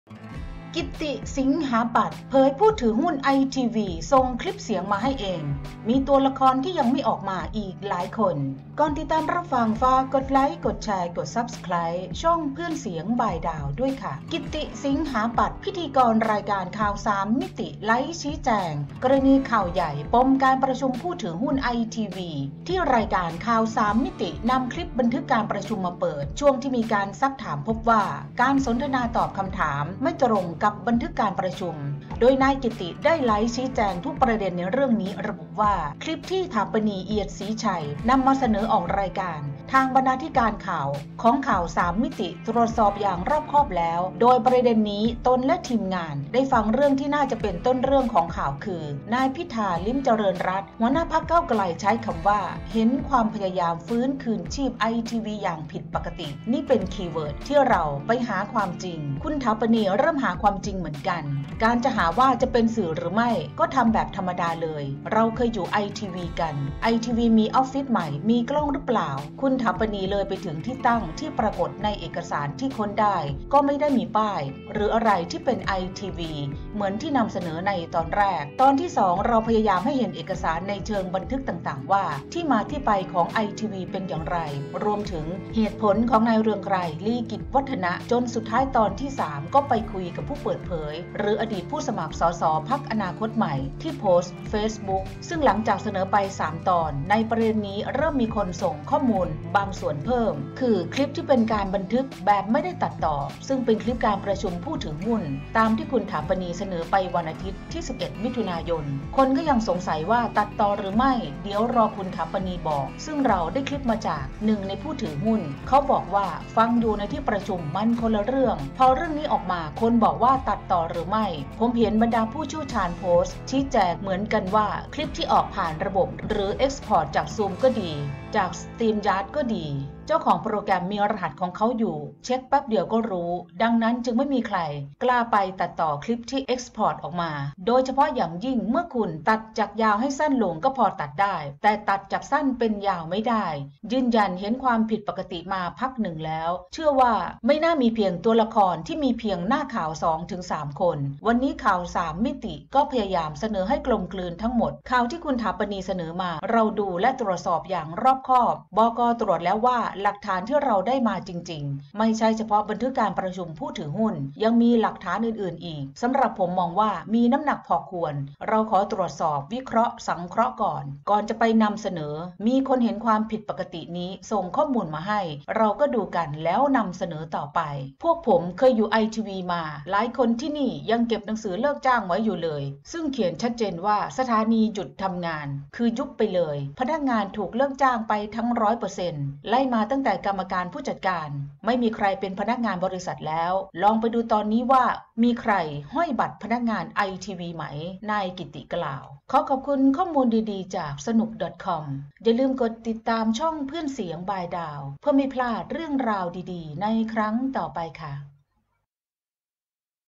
"กิตติ สิงหาปัด" เผย ITV หลักฐานยังมีอีก มีตัวละครที่ยังไม่ออกมาอีกหลายคน
หนังสือเสียง สำหรับทุกคนที่สนใจ
ขอบคุณถาพเรื่องจาก sanook.com/
ภาพจากเจ้าของภาพทุกภาพ  sanook.com